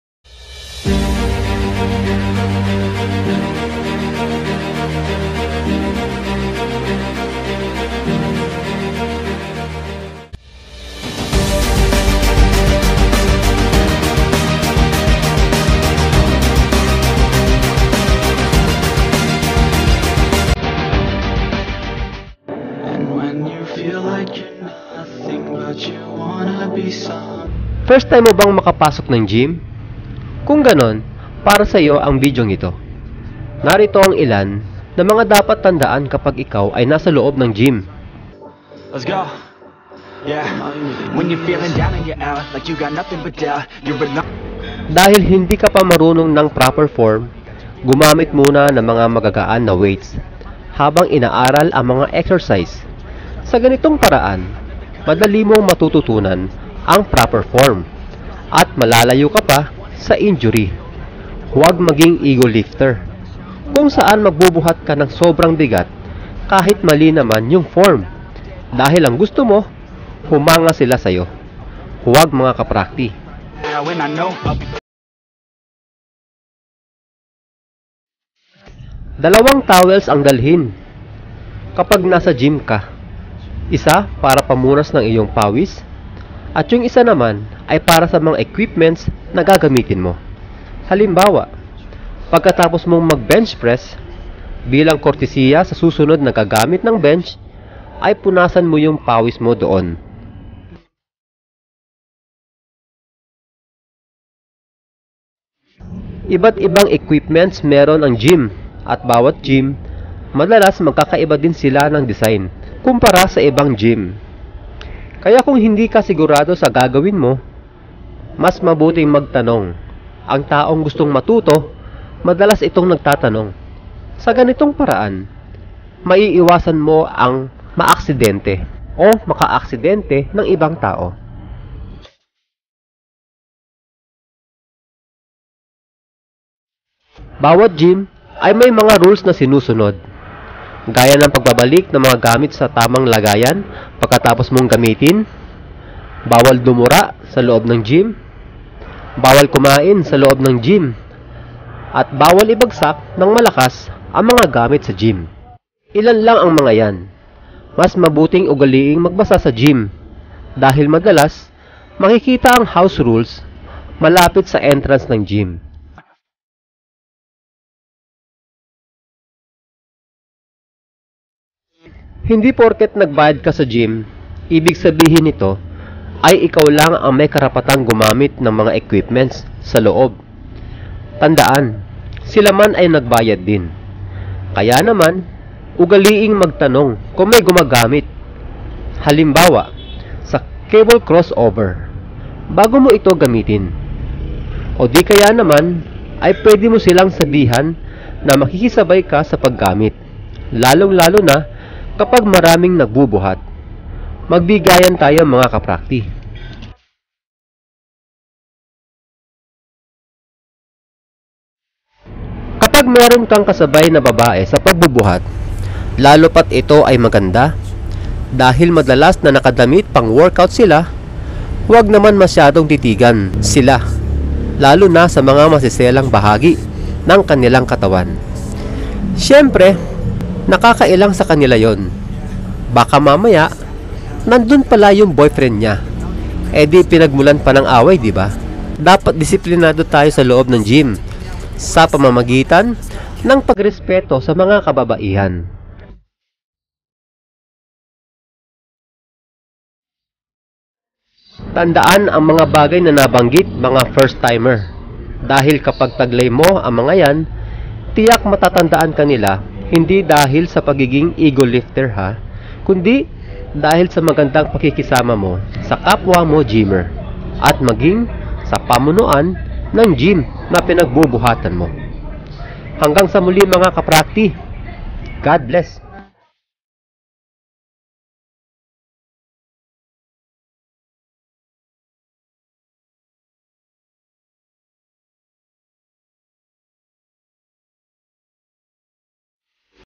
FIRST TIME SA GYM? MGA DAPAT AT DI DAPAT GINAGAWA SA GYM!
Kung ikaw ay baguhan sa gym, dapat mong alamin ang mga bagay na ito na nasa video. Upang maiwasan nating ma-Ban sa anumang Gym na papasukin natin.

intro music background
KA photography